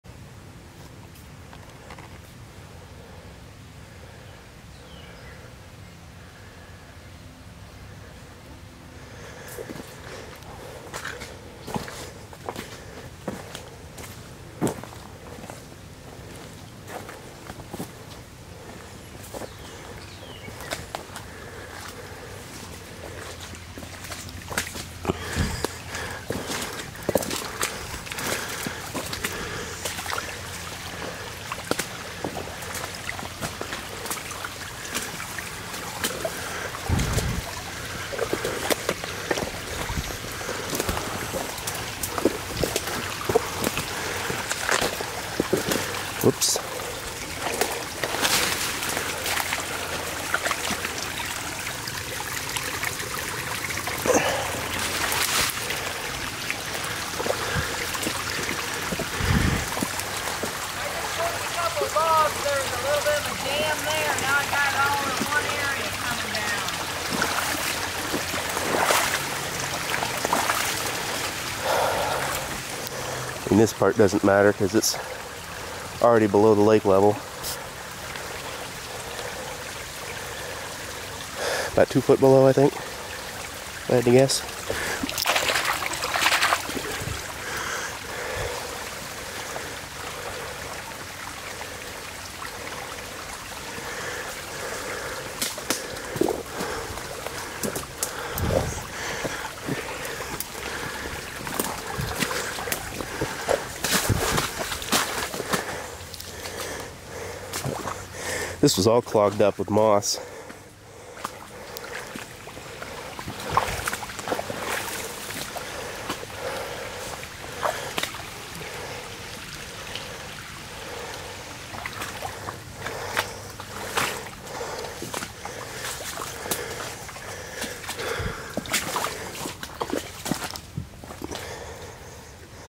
Checkout out spillway after heavy rains
Spillway was dug out a few days earler after we noticed the south side of the levee/dam was being topped, now that the water level has dropped even after heavy rains we were able to check out the rest of the spillway